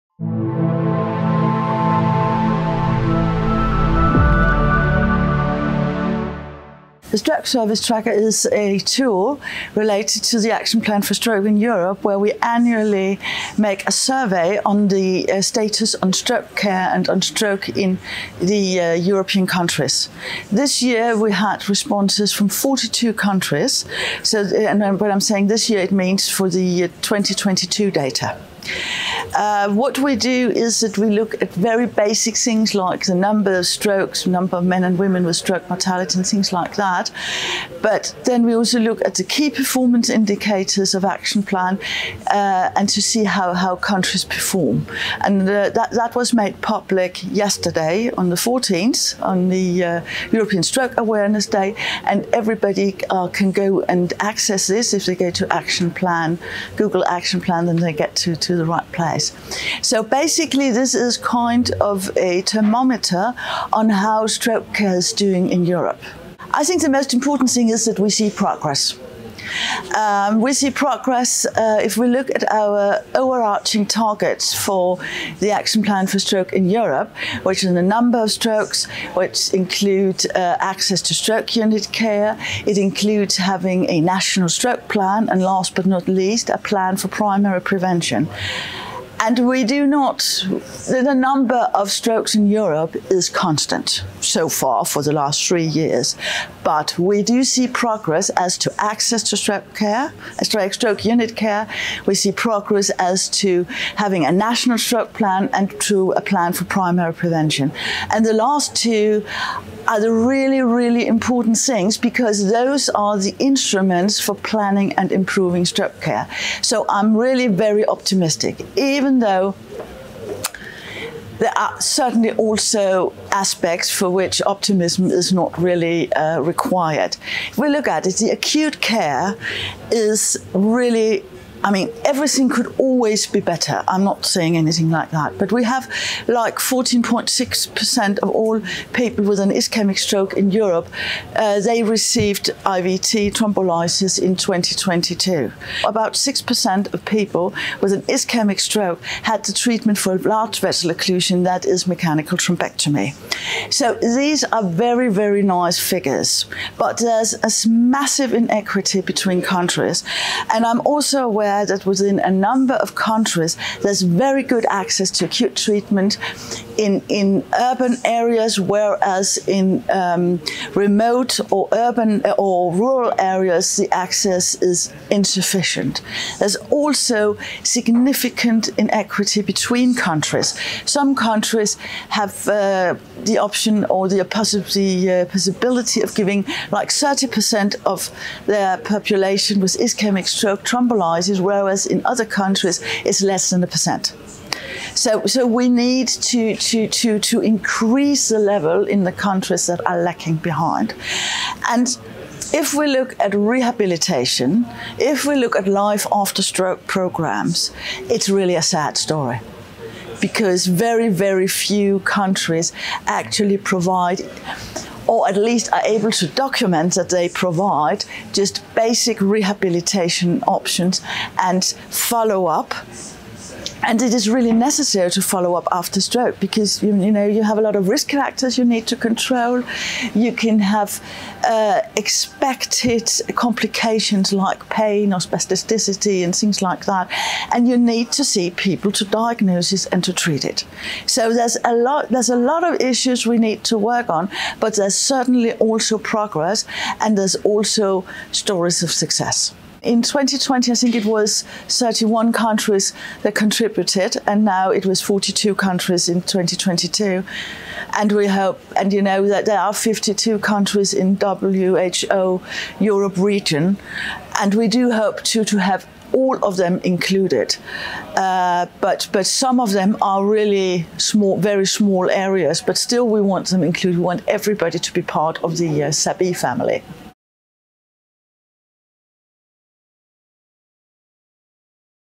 Stroke Service Tracker data shows major successes and challenges in European stroke care
The Stroke Service Tracker was launched as part of the Stroke Action Plan for Europe (SAP-E), collecting data on stroke incidence, mortality, stroke care pathways, and other key performance indicators. It aims to monitor the progress of quality of care and outcomes, and allow for comparisons among participating countries. In this video, Hanne Christensen, MD, PhD, MSci, University of Copenhagen, Copenhagen, Denmark, discusses the 2022 data which has just been published. While stroke incidence in Europe remained constant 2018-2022, improvements were seen in access to stroke unit care and in having a national stroke plan and a plan for primary prevention. The data also highlighted significant inequity between countries regarding access to thrombolysis and thrombectomy. Prof. Christensen highlights rehabilitation and post-stroke follow-up as one of the areas of greatest need, with very few countries providing adequate care. This interview took place at the 10th European Stroke Organisation Conference (ESOC) 2024 in Basel, Switzerland.